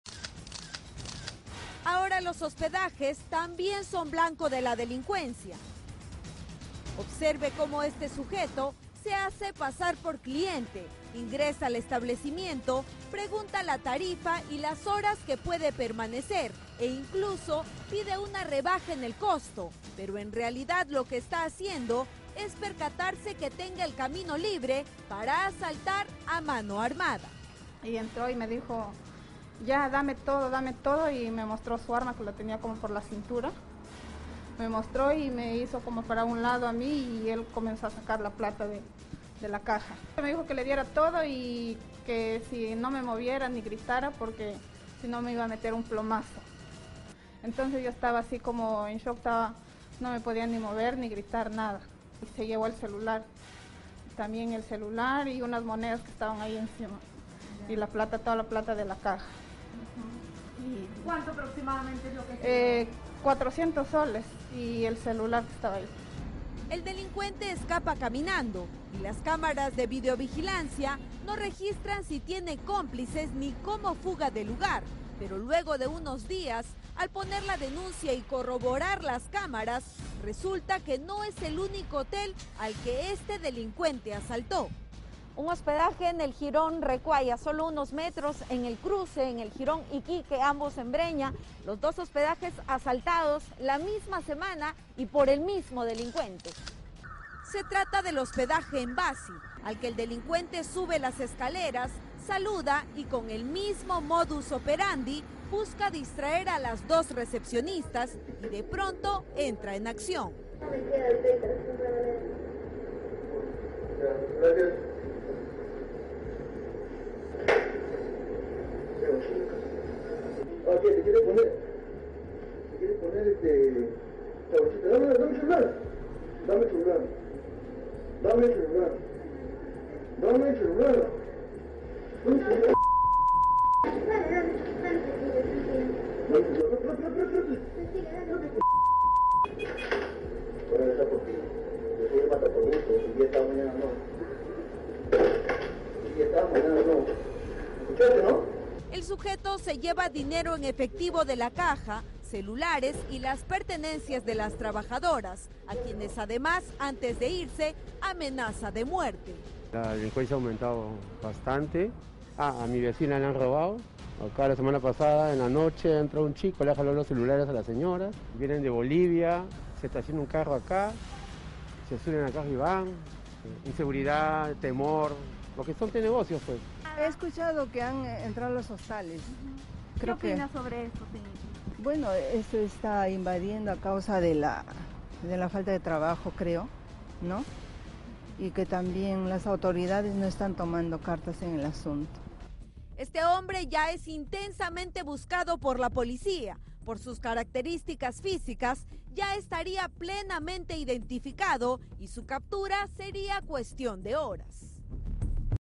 Breña: delincuente asalta dos hostales en una misma cuadra
En el distrito de Breña un sujeto se hizo pasar como cliente para robar en un hostal. Primero preguntó por una habitación y luego aprovechó el descuido de la trabajadora para ingresar a la recepción y encañonarla.

En el momento en el que traspasó el escritorio, la encañonó y en cuestión de segundos se llevó todo el dinero de la caja.

Pero no es el único hostal que ha sido asaltado por este delincuente. Otro de estos negocios también fue atacado por este mismo ladrón en la cuadra 4 de la avenida Iquique. Prácticamente el mismo modus operandi.